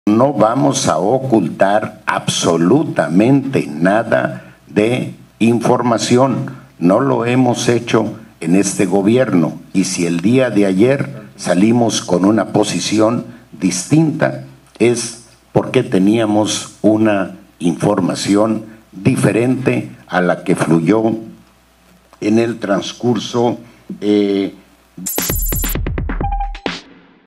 No vamos a ocultar información: Alfonso Durazo
Durante la rueda de prensa que ofreció el Gabinete de Seguridad Federal, Alfonso Durazo Montaño, dijo que no se ocultará información de los hechos ocurridos en Alfonso Durazo, Sinaloa.